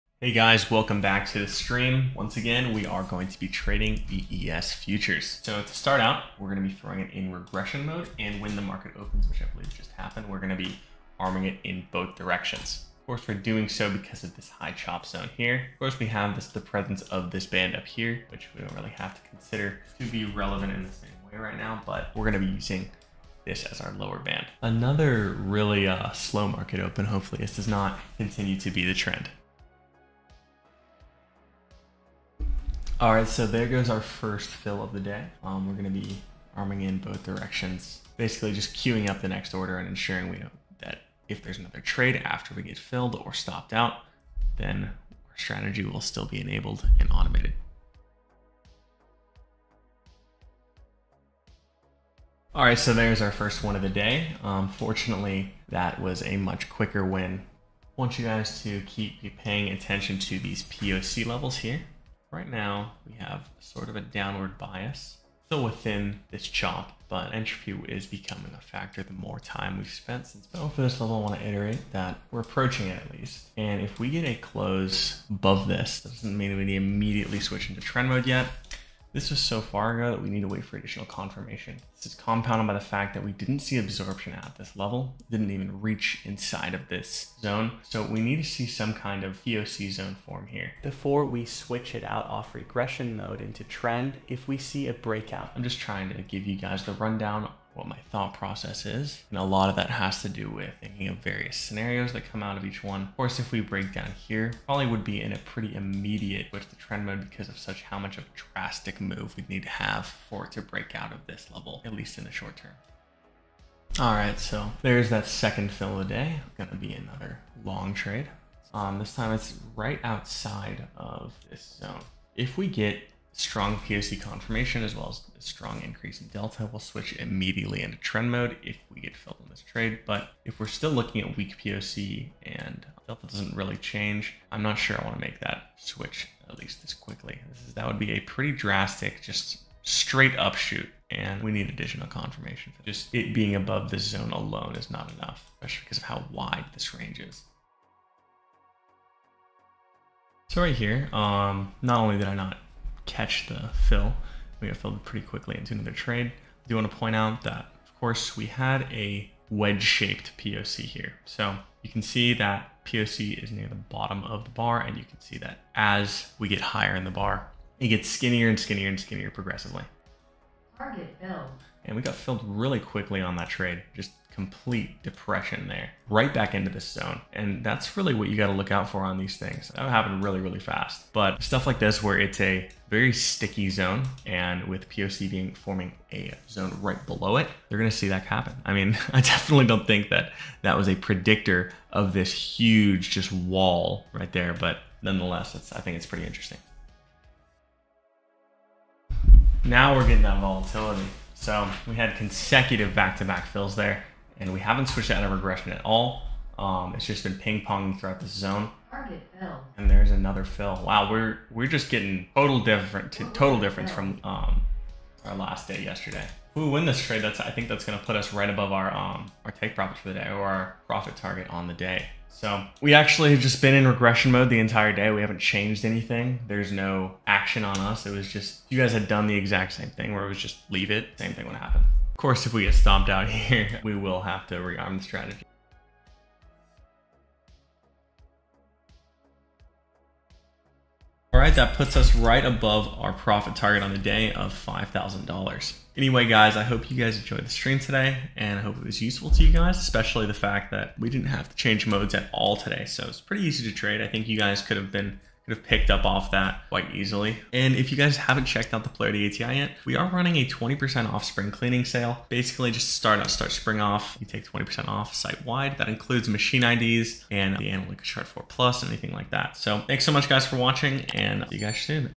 How I Scalped Ticks in 30 Minutes Trading Futures
Free Green and Blue Lines on Chart (SIGNUP FOR EMAIL AT BOTTOM OF PAGE)

ATM Settings:
12 Take Profit
12 Stop Loss

Polarity ATI and AnalyticaChart4 Settings:
0.35 Imbalance Ratio
75 Min Volume
3 Min imbalances
1 Tick Gap Trades Enabled
3 Strength
3 Offset
15 Range
Diagonal Imbalance Calculation Mode

60/40 Reverse Strength (Add two indicators to your chart)
14 period